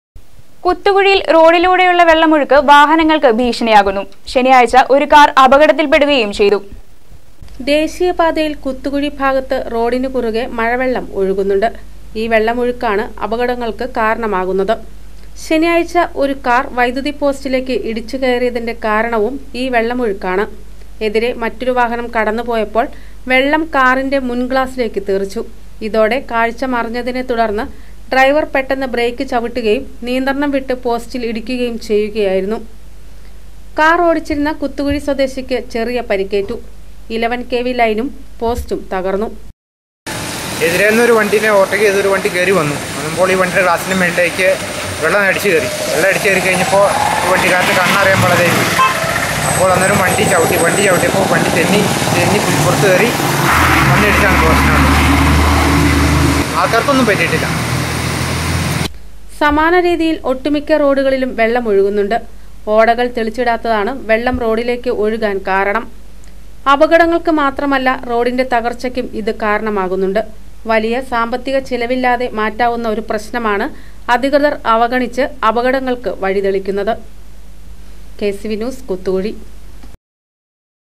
കുത്തുകുഴിയില്‍ റോഡിലൂടെയുളള വെള്ളമൊഴുക്ക് വാഹനങ്ങള്‍ക്ക ഭീഷണിയാകുന്നു.
കുത്തുകുഴിയില്‍ റോഡിലൂടെയുളള വെള്ളമൊഴുക്ക് വാഹനങ്ങള്‍ക്ക ഭീഷണിയാകുന്നു.ശനിയാഴ്ച ഒരു കാര്‍ അപകടത്തില്‍പ്പെടുകയും ചെയ്തു.